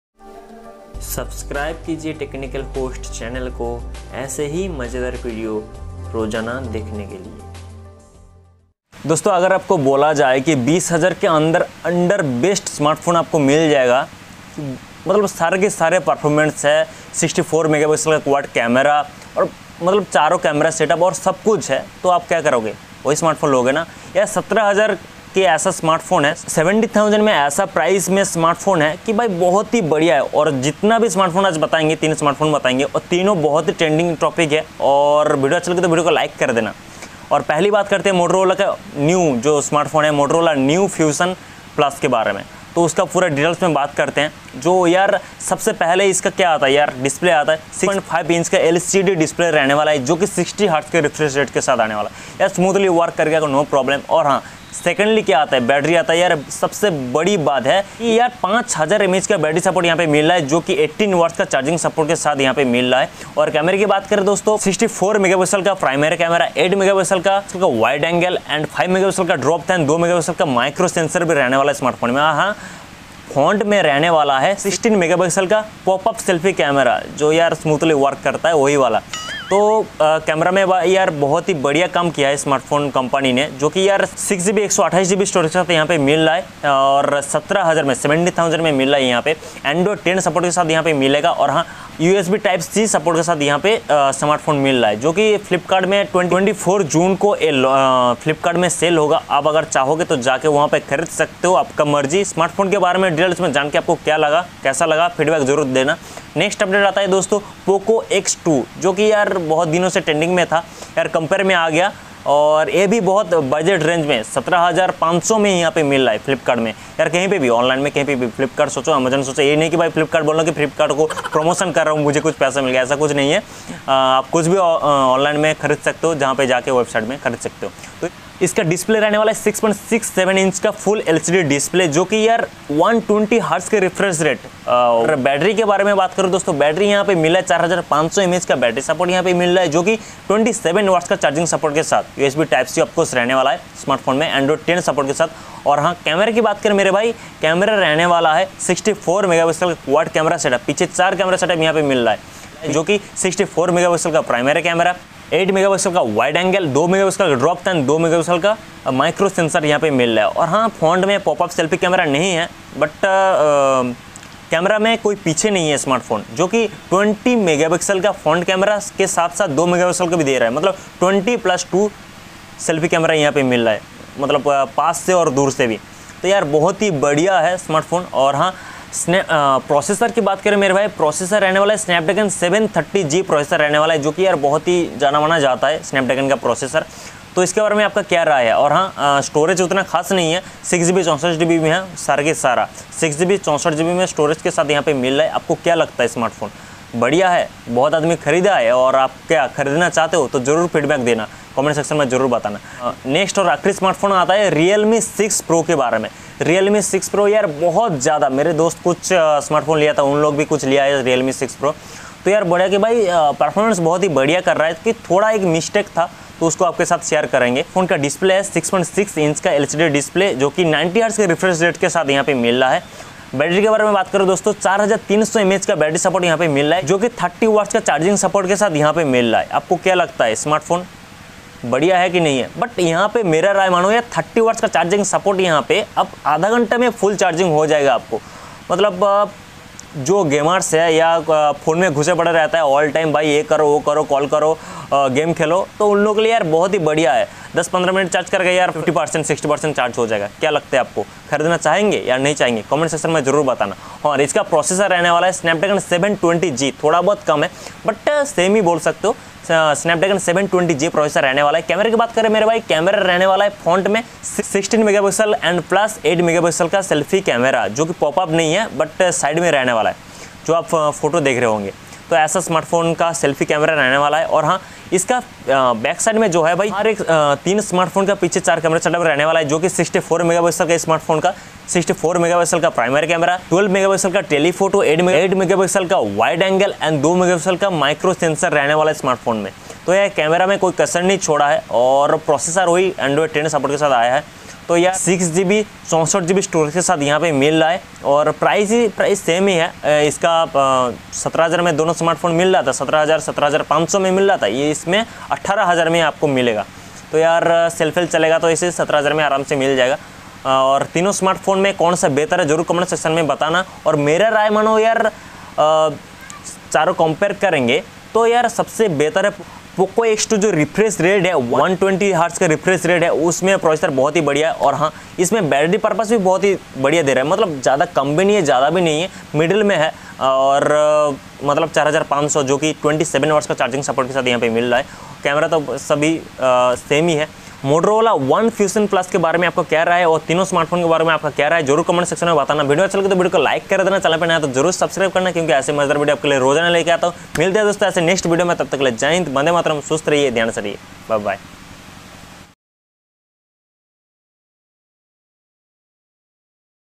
motorola ome fusion+ || poco X2 || realme 6pro || under 17k best phone
doston is video mein main realme 6 Pro Pogo X2 and Motorola one fution+ ke bare mein bataya hai

video timeline:-
00:09 video introduce
00:10 told video topics
00:36 Motorola one fution+ full details
01:36 Poco X2 full details
03:23 realme 6 pro full detail
05:10 video compare feedback for me
05:42 please send your feedback request this video
05:51 video end formality